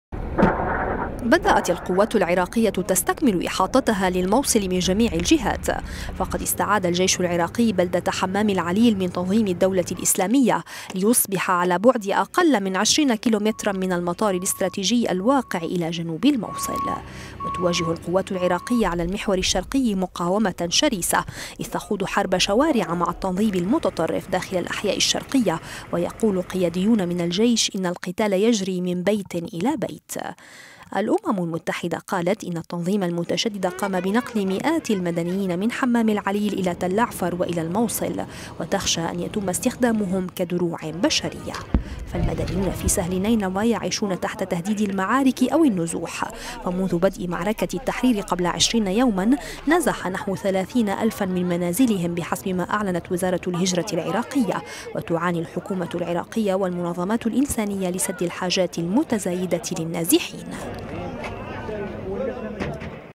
الجيش العراقي يستعيد "حمام العليل" جنوب الموصل - world
بدأت القوات العراقية تستكمل إحاطتها للموصل من جميع الجهات.

فقد استعاد الجيش العراقي بلدة حمام العليل من تنظيم الدولة الإسلامية ليصبح على بعد أقل من 20 كيلومتراً من المطار الاستراتيجي، الواقع إلى الجنوب من الموصل.

وأكد مصدر من الجيش أنهم بدؤوا بتمشيط البلدة بحثاً عن مسلحي التنظيم والمفخخات التي خلفوها.

وتواجه القوات العراقية على المحور الشرقي مقاومة شرسة، إذ تخوض حرب شوارع مع التنظيم المتطرف، داخل الأحياء الشرقية، ويقول قياديون في الجيش إن "القتال يجري من بيت إلى بيت".